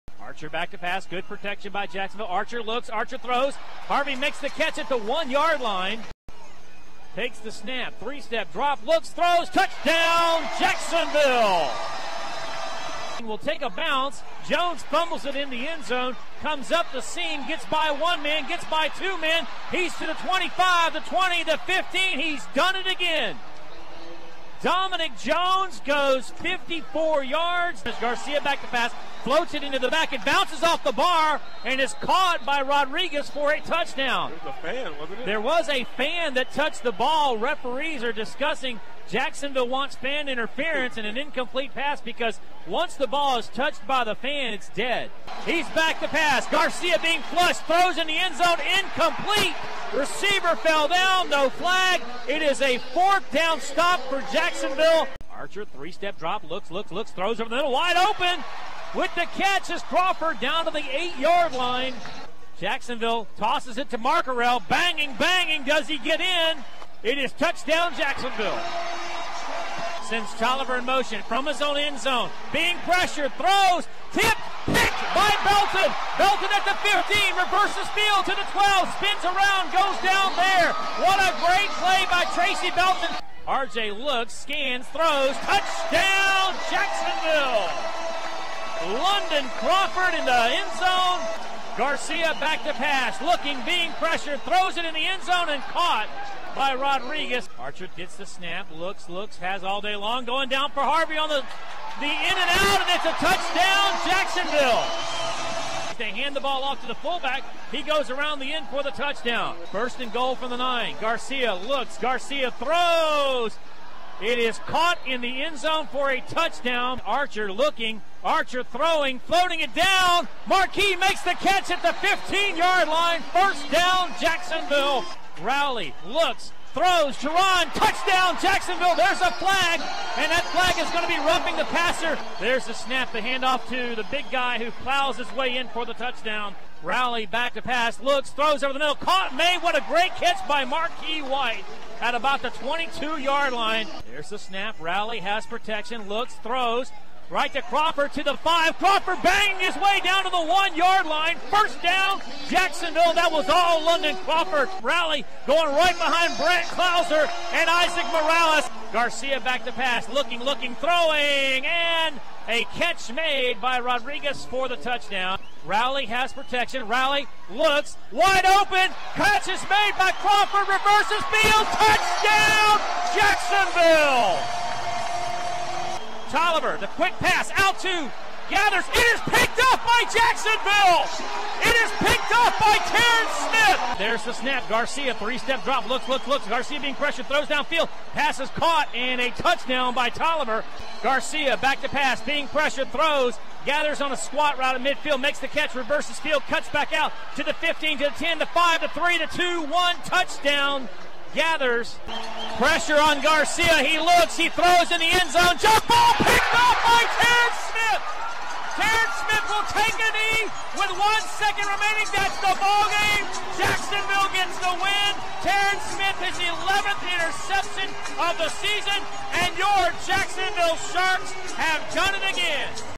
Jacksonville Sharks 62, Orlando Predators 55
The Jacksonville Sharks defeated the Orlando Predators, 62-55, on Sea Best Field at the Jacksonville Veterans Memorial Arena on Saturday, June 29, 2013.